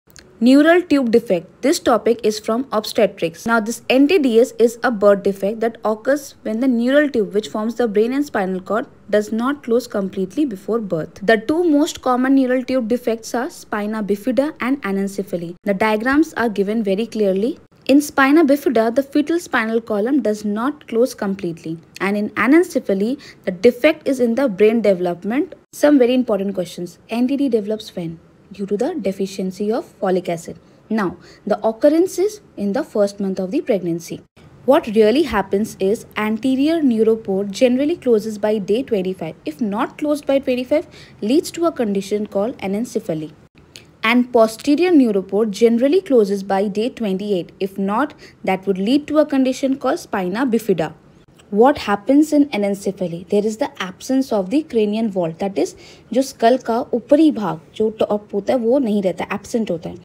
Neural tube defects
The most common birth-defect in India is

  The Neural Tube Defect may vary from Anencepaly, spina bifida occulta and spina bifida cystica.

  Neural tube defects are birth defects of the brain, spine, or spinal cord. It happens in the first month of pregnancy, often before a woman even knows that she is pregnant.

 The two most common neural tube defects are Spinabifida AND ANENCEPHALY.

 In Spina bifida, the fetal spinal column doesn't close completely.

 In Anencephaly defect in brain development occur.